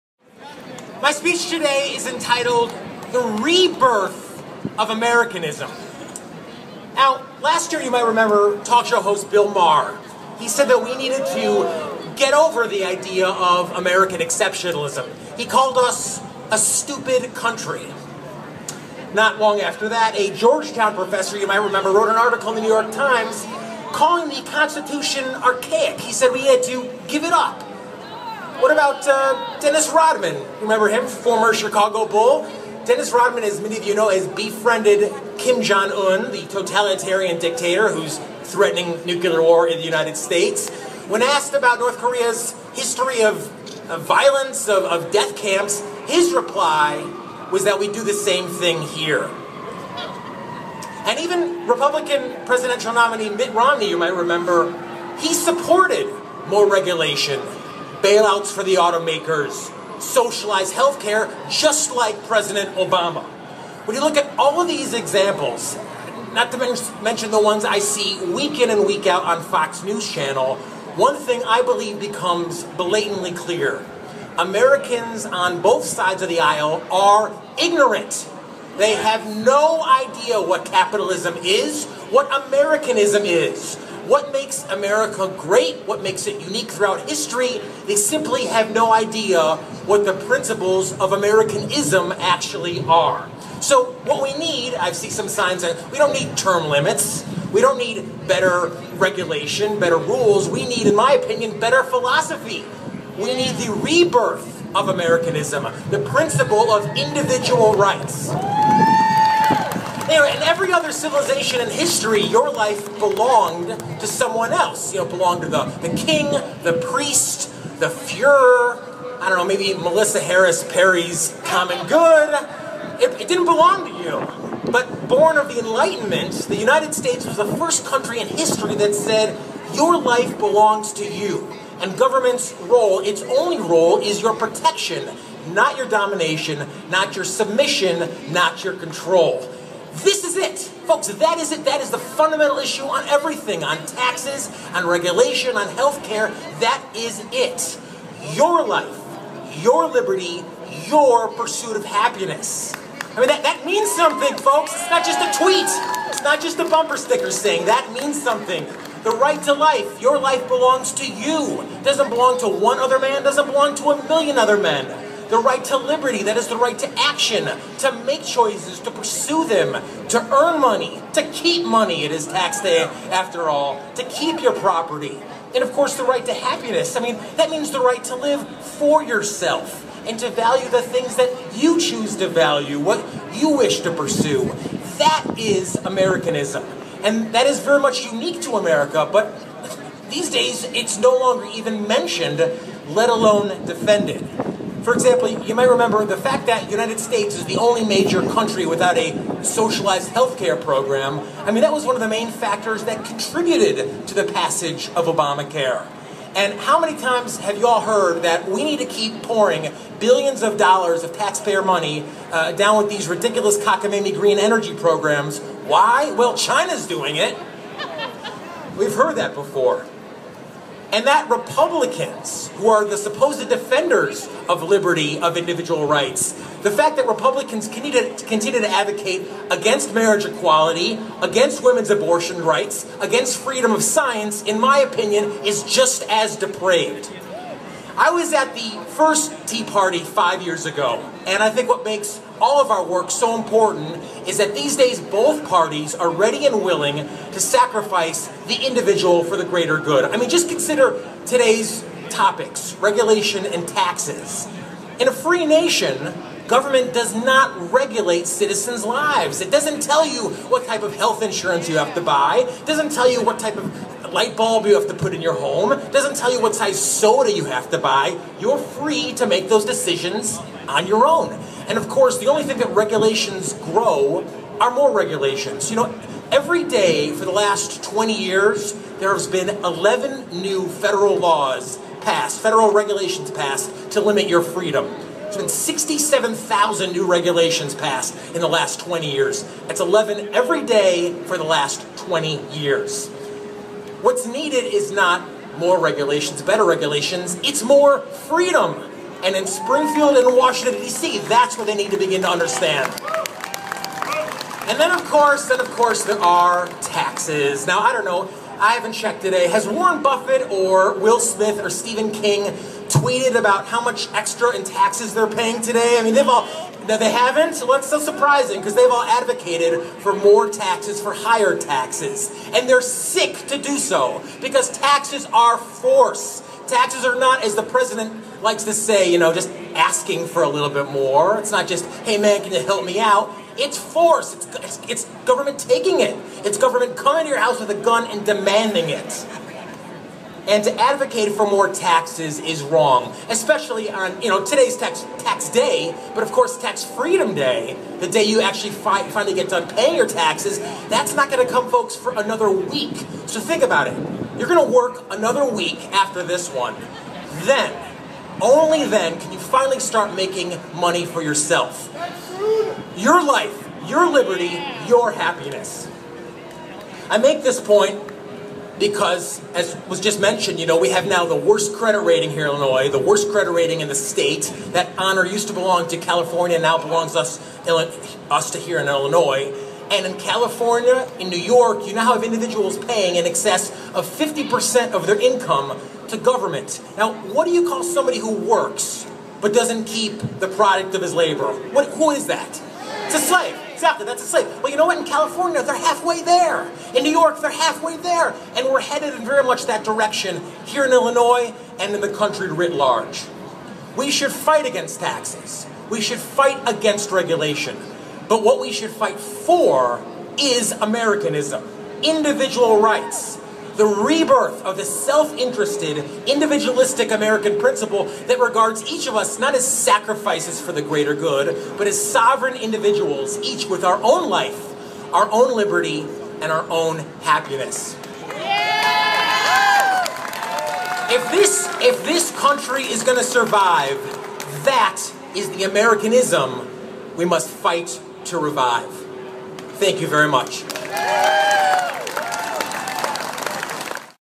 Renascimento do Americanismo/individualismo | Jonathan Hoenig
No vídeo de hoje, Jonathan Hoenig reflete sobre os valores do Americanismo, dos valores que balizaram os Estados Unidos.